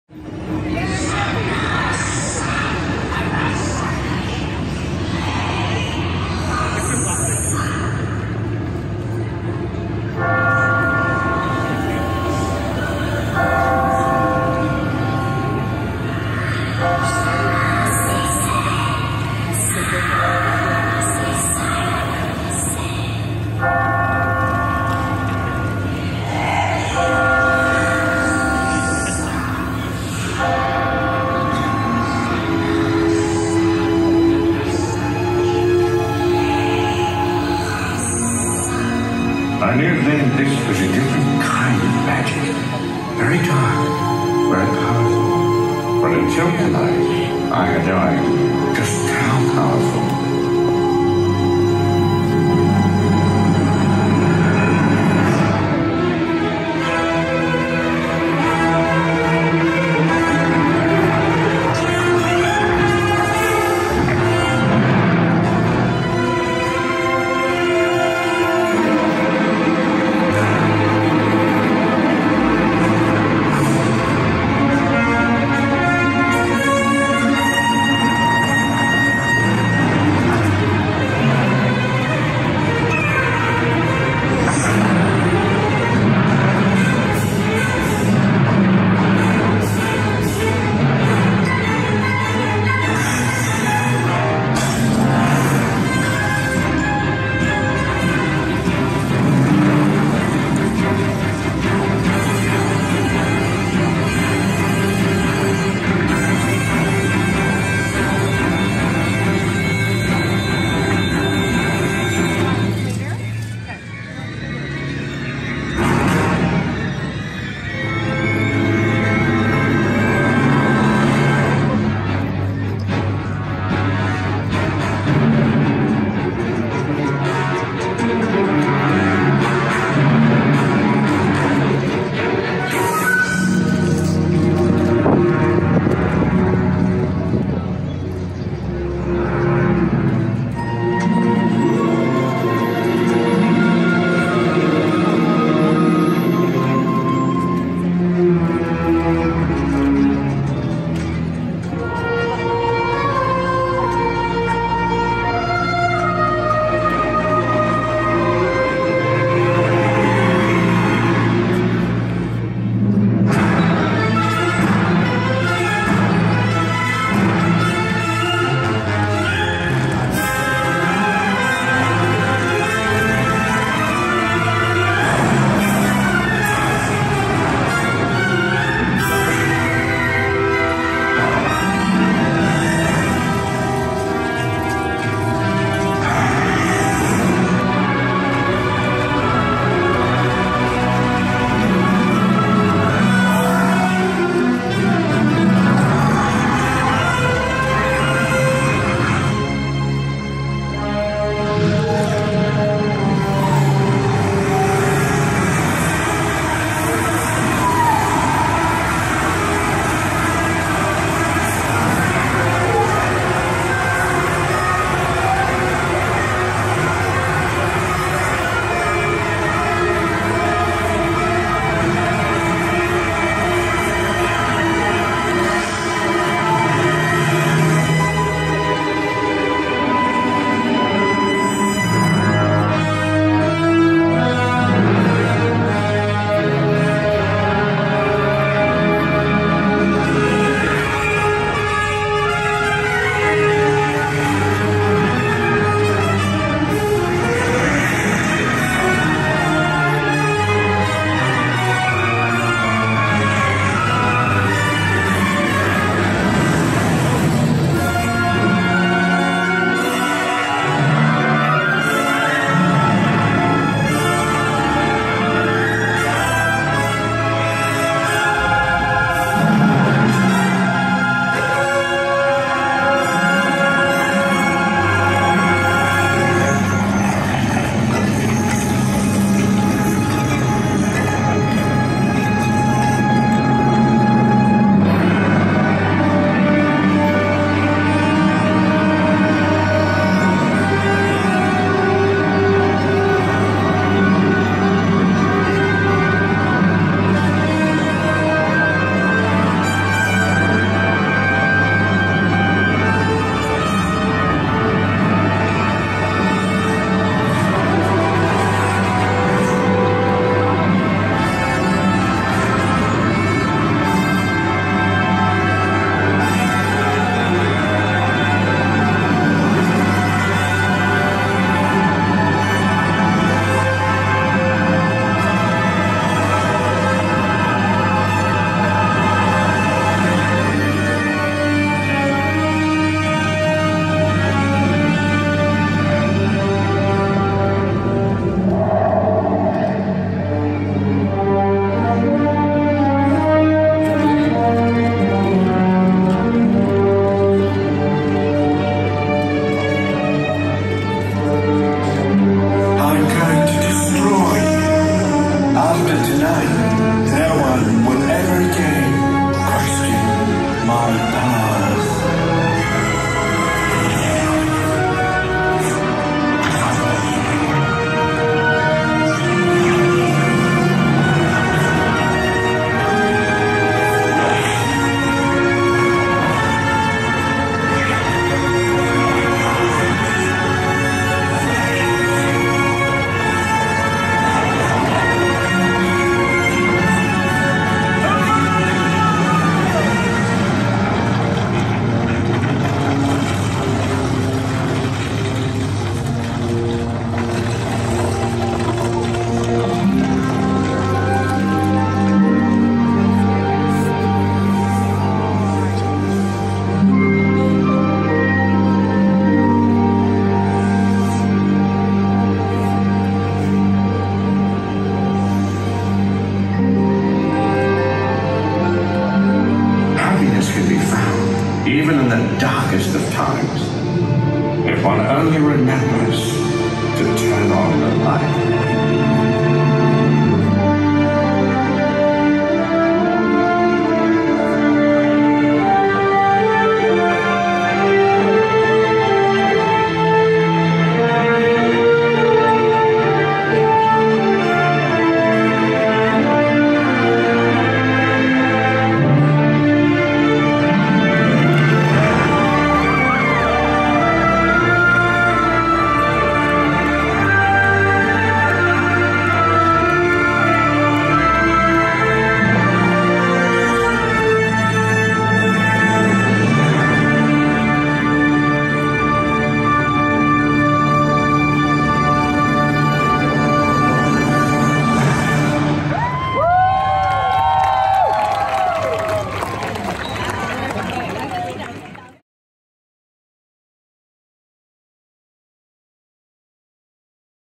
4K “Dark Arts at Hogwarts Castle” Projection Show | Universal Studios Hollywood
Full 4K “Dark Arts at Hogwarts Castle” projection show at Universal Studios Hollywood. This was the last showing until end of May.

As darkness falls at Hogwarts™ castle, Dementors™, Death Eaters™ and other unspeakable creatures rise to cloak the stone walls in sinister images and fiendish magic. Chaos erupts as the image of Lord Voldemort™ joins the fray, summoning his cruel legions to swarm the castle. Experience the fire-engulfed army of Inferi and the soul-draining breath of Dementors™ as you stare in awe at the castle’s takeover. Accompanied by an atmospheric music score created by John Williams, this powerful spectacle is an unforgettable end to your day.

Preview it now during the select nights below:

April 13-28
May 25-27